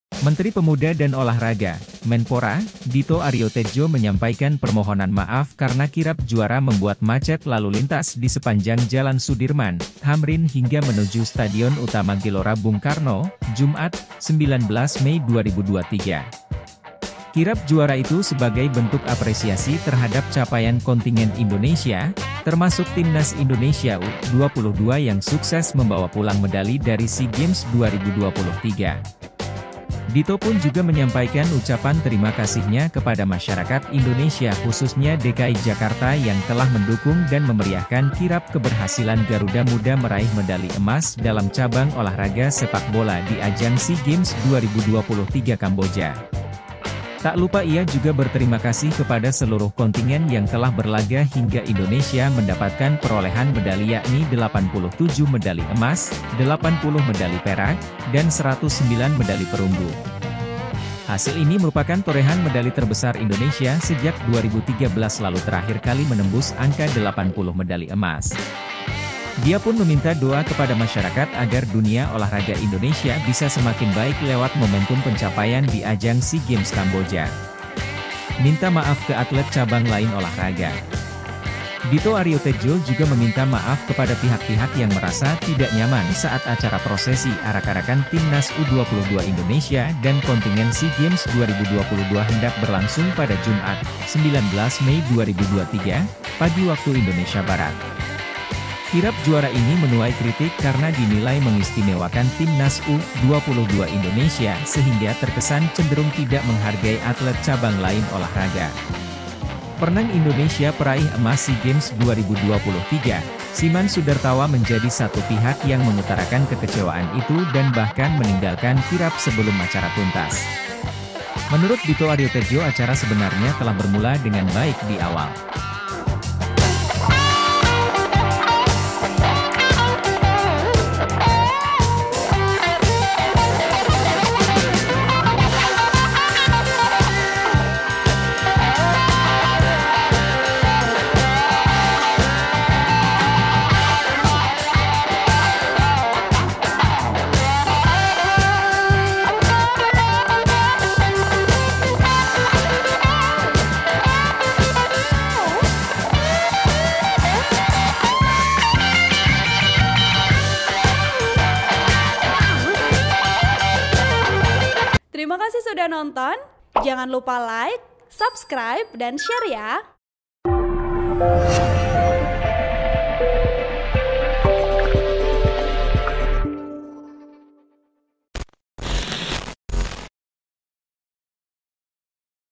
Menpora Minta Maaf Buat Atlet Lain Merasa Tidak Dihargai dan Kirab Juara Bikin Jalanan Macet
TRIBUN-VIDEO.COM, JAKARTA - Menteri Pemuda dan Olahraga (Menpora), Dito Ariotedjo menyampaikan permohonan maaf karena Kirab Juara membuat macet lalu lintas di sepanjang Jalan Sudirman - Thamrin hingga menuju Stadion Utama Gelora Bung Karno, Jumat (19/5/2023).

Kirab Juara itu sebagai bentuk apresiasi terhadap capaian kontingen Indonesia, termasuk Timnas Indonesia U-22 yang sukses membawa pulang medali dari SEA Games 2023.

"Terima kasih juga untuk seluruh masyarakat Indonesia khususnya Jakarta, mohon maaf hari ini 2 jam kita bikin macet tapi energinya sangat positif," kata Dito dalam sambutannya di GBK, Senayan, Jakarta Pusat, Jumat.

Dito pun juga menyampaikan ucapan terima kasihnya kepada masyarakat Indonesia khususnya DKI Jakarta yang telah mendukung dan memeriahkan kirab keberhasilan Garuda Muda meraih medali emas dalam cabang olahraga sepak bola di ajang SEA Games 2023 Kamboja.

Tak lupa ia juga berterima kasih kepada seluruh kontingen yang telah berlaga hingga Indonesia mendapatkan perolehan medali yakni 87 medali emas, 80 medali perak, dan 109 medali perunggu.

Hasil ini merupakan torehan medali terbesar Indonesia sejak 2013 lalu terakhir kali menembus angka 80 medali emas.

Dia pun meminta doa kepada masyarakat agar dunia olahraga Indonesia bisa semakin baik lewat momentum pencapaian di ajang SEA Games Kamboja.

"Dan juga kawan - kawan yang ada di GBK hari ini saya ucapkan terima kasih sudah mendukung seluruh kontingen Indonesia dan mendoakan kita mendapatkan juara sepak bola setelah 32 tahun dan juga perolehan medali kita melesat ke atas menjadi 87 untuk emas, 80 untuk silver, dan 109 perunggu," ungkapnya.

"Sekali lagi saya mohon doanya untuk dunia olahraga kita," kata dia.

Minta Maaf ke Atlet Cabang Lain Olahraga

Dito Ariotedjo juga meminta maaf kepada pihak-pihak yang merasa tidak nyaman saat acara prosesi arak-arakan timnas U22 Indonesia dan kontingen SEA Games 2022 hendak berlangsung pada Jumat (19/5/2023) pagi WIB.

Kirab Juara ini menuai kritik karena dinilai mengistimewakan Timnas U-22 Indonesia sehingga terkesan cenderung tidak menghargai atlet cabang lain olahraga.

Perenang Indonesia peraih emas SEA Games 2023, Siman Sudartawa menjadi satu pihak yang mengutarakan kekecewaan itu dan bahkan meninggalkan kirab sebelum acara tuntas.

Menurut Dito Ariotedjo acara sebenarnya telah bermula dengan baik di awal.

Ia mengapresiasi persiapan di halaman gedung Kemenpora yang menurutnya sangat seru dan menyenangkan karena ada beberapa artis yang datang seperti JKT 48, Marshel, dan juga Dustin.

"Memang karena menunggu atlet bola tadi akhirnya kita putuskan langsung mulai acaranya," ujar Dito seperti dikutip dari BolaSport.

"Ternyata ada kekecewaan dari atlet renang Siman (Sudartawa). Tapi, ini tidak juga kita berniat seperti itu."

Dito mengutarakan bahwa pihaknya berinisiatif untuk mengadakan acara yang baru pertama kali bergulir ini.

Apalagi, kontingen Indonesia menutup SEA Games 2023 di urutan ketiga klasemen dengan perolehan 87 medali emas, 80 perak, dan 109 perunggu.

"Justru kita hari ini mencoba untuk mengapresiasi baru pertama kalinya atlet SEA Games ini kita apresiasi untuk pawai di Jakarta," tuturnya menambahkan.

"Menurut saya di SEA Games ini banyak sekali target-target yang sudah lama tidak terlampaui, contohnya dari jumlah medali emas, ini adalah perolehan emas terbanyak selama 20 tahun terakhir."

Ia memuji keberhasilan di mana kontingen Indonesia menjadi juara umum di tujuh cabang olahraga dengan empat atau lima mencetak sejarah seperti timnas basket putri yang mendapatkan emas pertama kali dan timnas sepak bola putra meraih medali emas setelah 32 tahun.

Tak hanya itu, hoki putra juga mendapat emas, kriket pun sama, dan atletik mendapatkan emas pertama di nomor 4x100 meret setelah 12 tahun.

Namun, keberhasilan timnas U22 Indonesia mendapatkan emas bisa dibilang paling menyita perhatian publik Tanah Air.

Sehingga, pihak Kemenpora berusaha menggabungkan antusiasme masyarakat kepada sepak bola dengna mempromosikan cabor-cabor lain.

"Ini budaya pertama di mana kita bisa dibilang memang menunggangi antusias masyarakat kepada bola dengan mempromosikan juga bahwa cabor-cabor lain juga berprestasi," tutur Dito.

"Memang pasti ada yang tidak nyaman dan kita sangat mohon maaf. Semoga ini jadi budaya baru yang nantinya juga cabor-cabor itu bisa mengikuti apa yang tadi PSSI lakukan."

"Jadi hari ini acaranya memang kKemenpora host. Pelepasannya dan PSSI kontribusi di satu titik waktu di GBK."

"Saya harap cabor-cabor lainnya bisa mengikuti dan mengapresiasi atlet-atletnya ketika pulang dari internasional event untuk bisa disemarakkan," tutupnya.

(Danang Triatmojo/Tribunnews/Firzie A. Idris/Kompas.com)

Video Production: Tri Susilo Mardhani